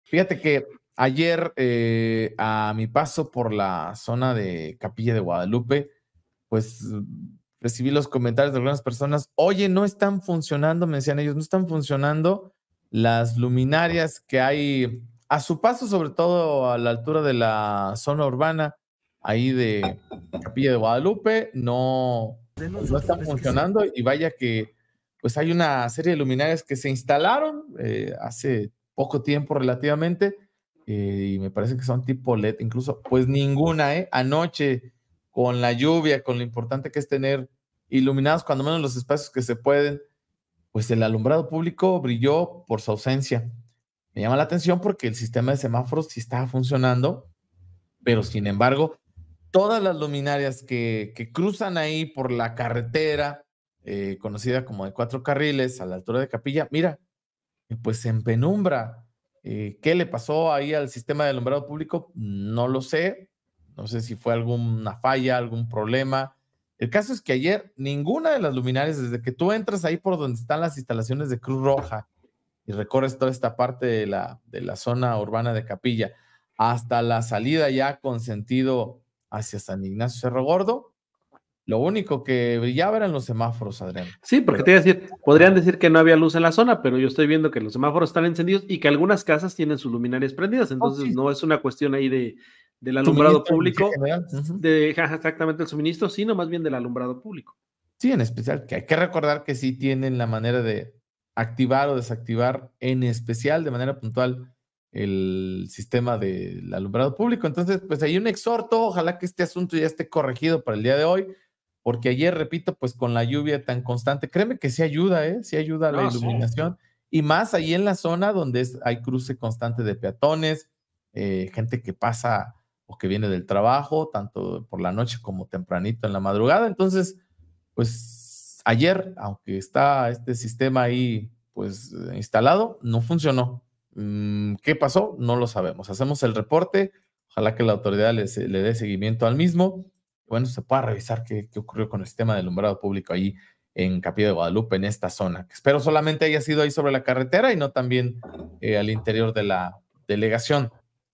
Reportan fallas de alumbrado público en Capilla de Guadalupe
Habitantes de la delegación de Capilla de Guadalupe, reportaron este martes por la noche fallas en el alumbrado público en la zona de la carretera, ninguna de las luminarias estaban funcionando.